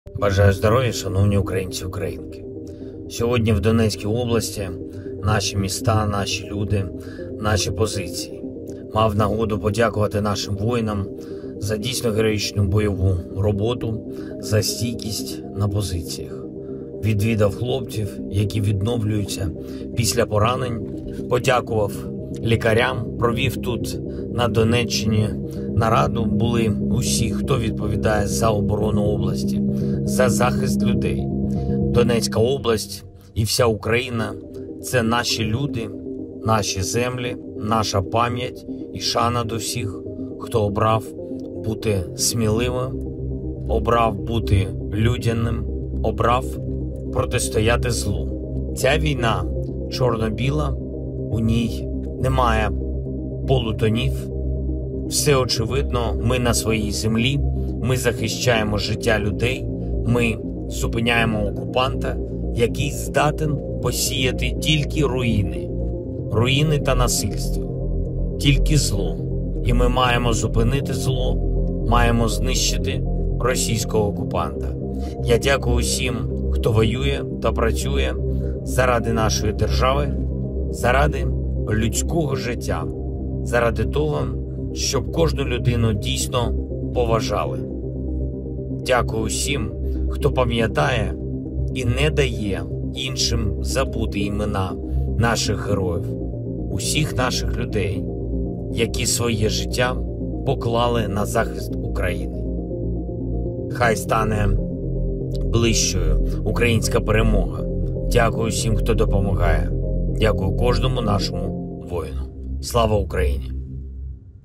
Звернення Володимира Зеленського за підсумками робочої поїздки на Донеччину
«Ця війна – чорно-біла, у ній немає напівтонів, усе очевидно. Ми – на своїй землі. Ми захищаємо життя людей. Ми зупиняємо окупанта, який здатен посіяти тільки руїни та насильство, тільки зло. І ми маємо зупинити зло. Маємо знищити російського окупанта. Я дякую всім, хто воює й працює заради нашої держави, заради людського життя, заради того, щоб кожну людину справді поважали».